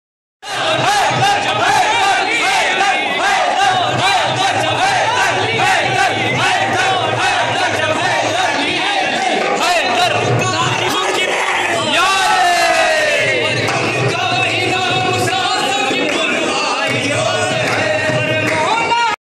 Nara e haidri at khanqah baqaiyah safipur sharif
A part of juloos e maula e kayenat at safipur sharif